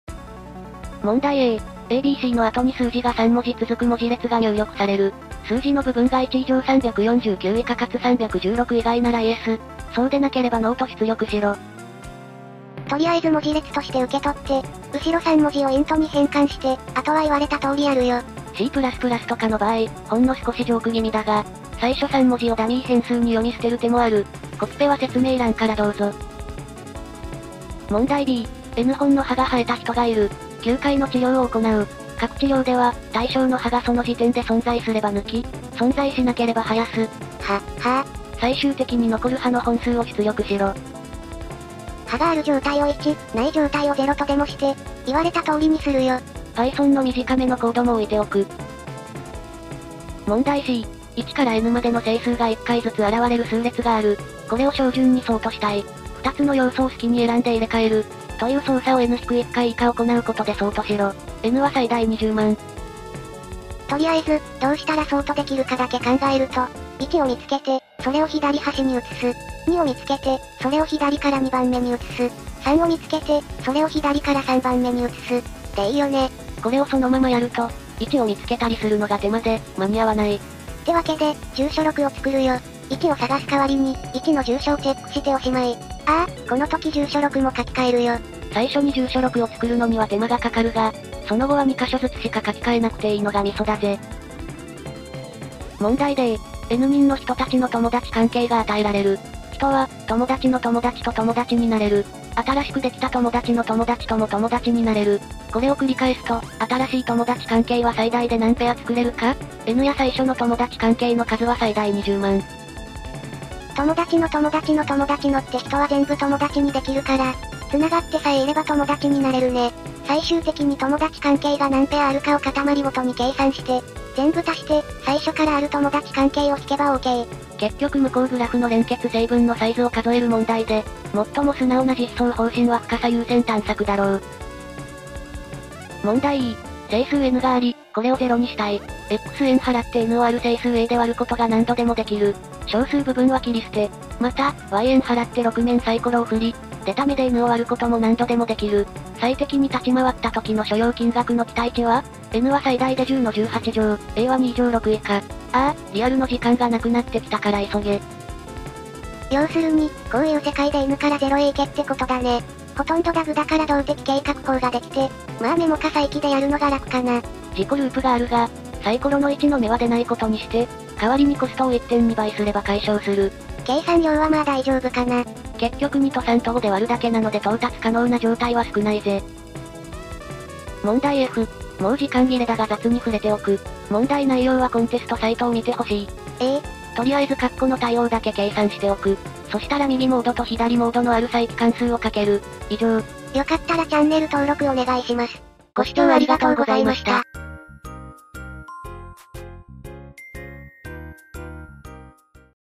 3分でAtCoder Beginner Contest 350 A-F【ゆっくり解説】
とあるプログラミングコンテスト（競技プログラミング）の高速解説です。
コンテストに取り組まずにいきなりこの動画を見ても多分大丈夫です。

0:00 A
0:27 B
0:50 C
1:35 D
2:16 E
3:00 F